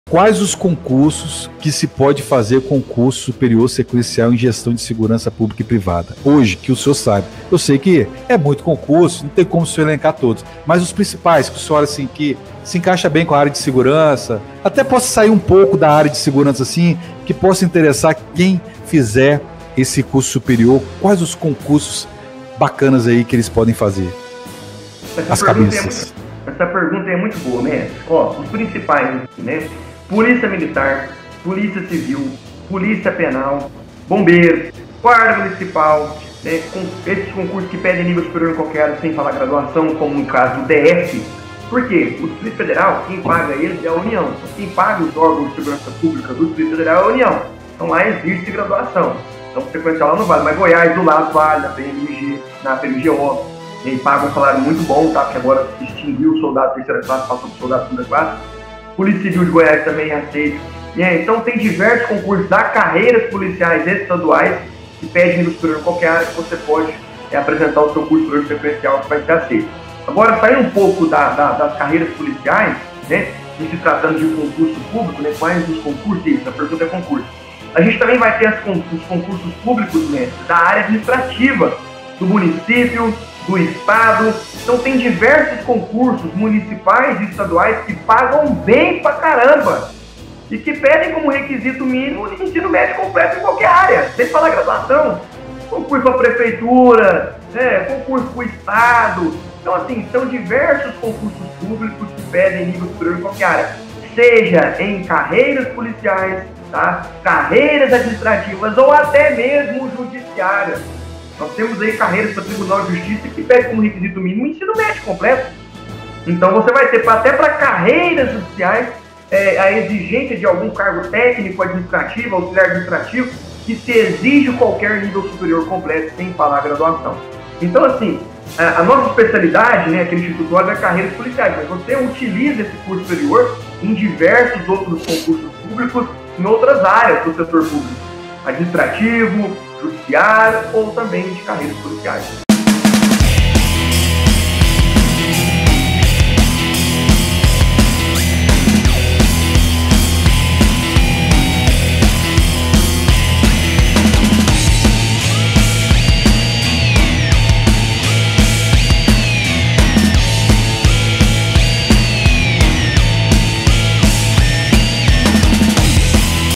Quais os concursos que se pode fazer com o Curso de Gestão ?
Com o curso Superior Sequencial de Gestão é permitido fazer algum concurso que saia ? Essa resposta da dúvida de muitos respondemos agora!

Faça seu curso Superior Sequencial em Gestao de Segurança Pública e Privada Agora nesse

🎥Curso Supervisor de Segurança - Operacional Administrativo
Outros Cursos para você fazer